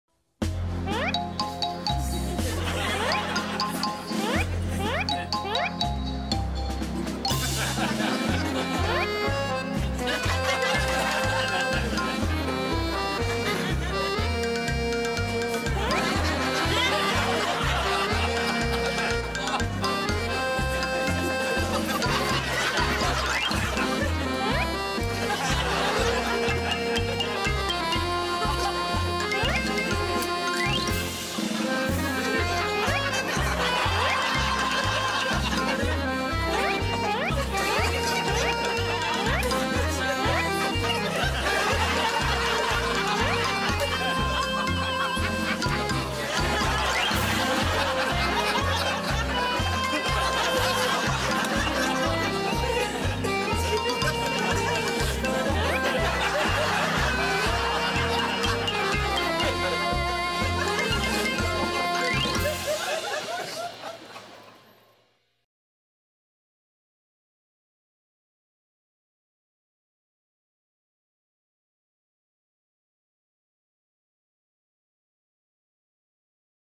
Vicious Plant Attack Caught On Camera
This vicious palm tree has no pity for its victims. It savagely gently rubs its leaves all over its victims terrorized faces and heads.